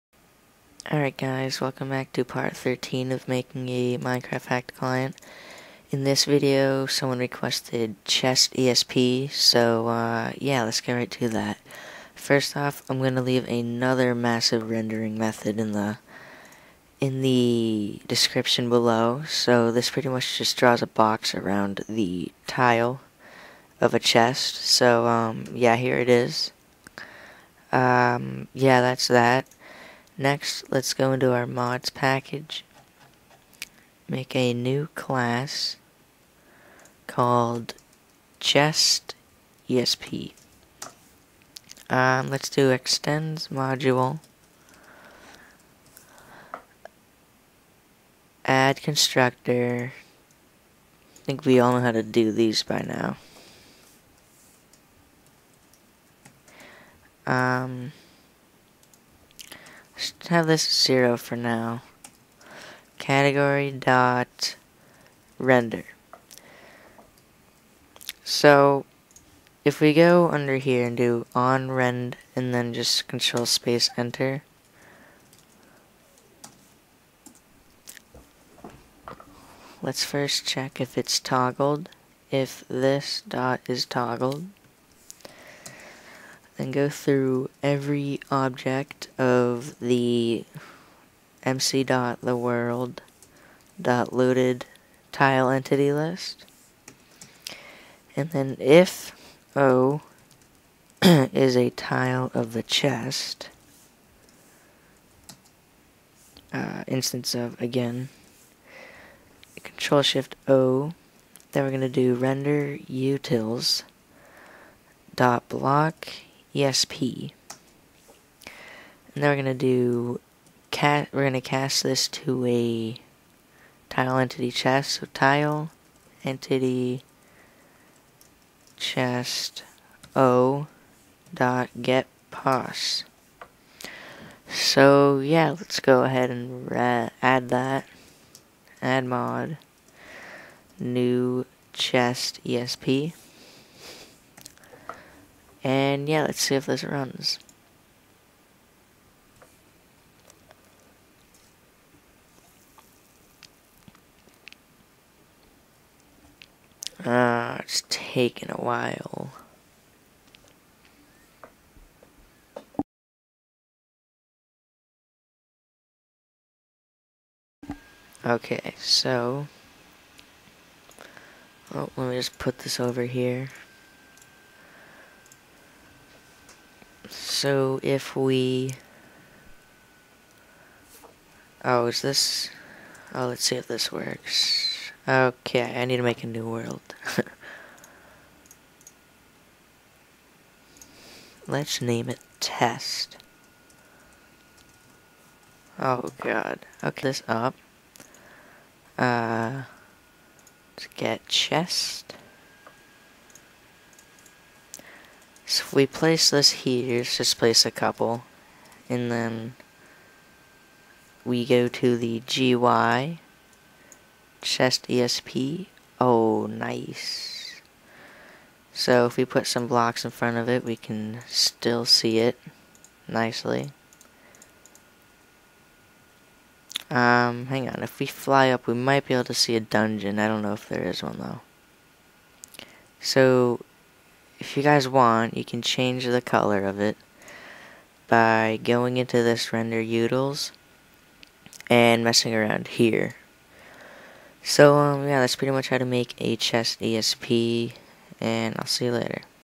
How To Code A Minecraft Hacked Client 1.8.8 #13 - ChestESP
In this video we will be creating a chest esp